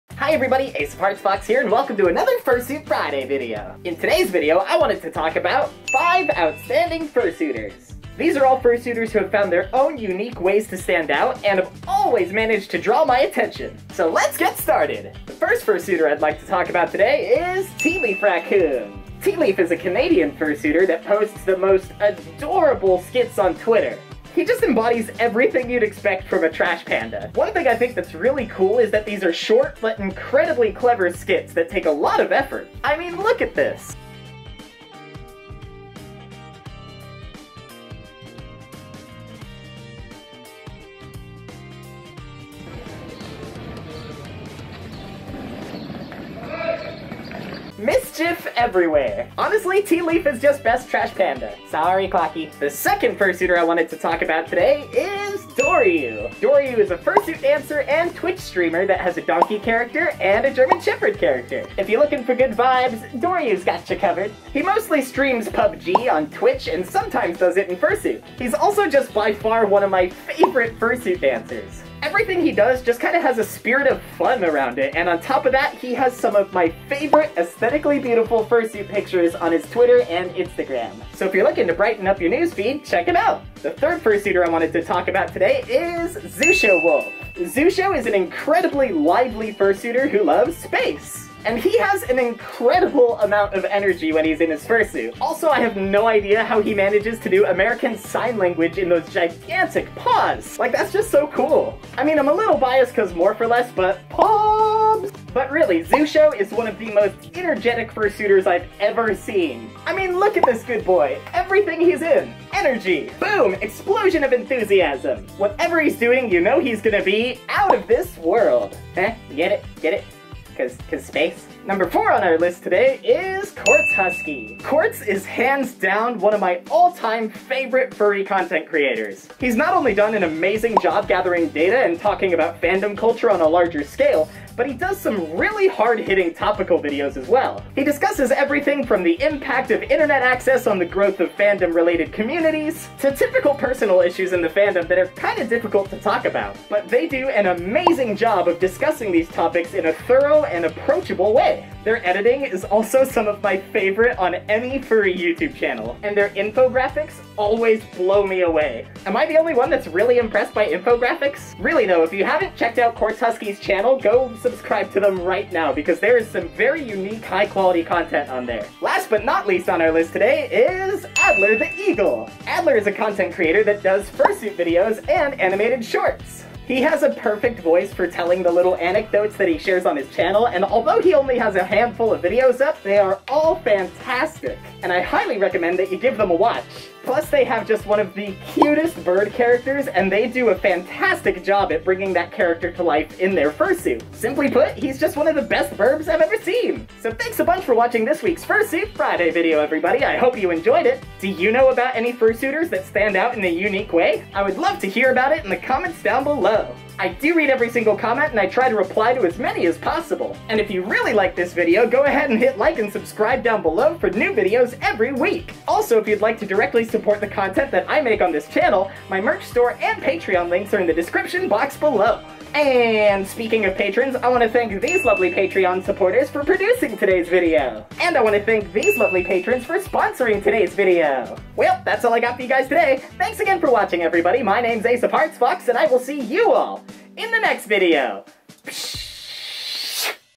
5 Outstanding Fursuiters!
Happy Fursuit Friday, everybody! Today, I wanted to spotlight some amazing fursuiters that found their own unique ways to stand out!

Each of these anthropomorphic animals has a special way of expressing their fursona, so I hope you enjoy today's vlog!

FOLLOW THESE AWESOME FURSUITERS ON TWITTER!

My fursuit was commissioned as a partial by More Fur Less. Please support their work!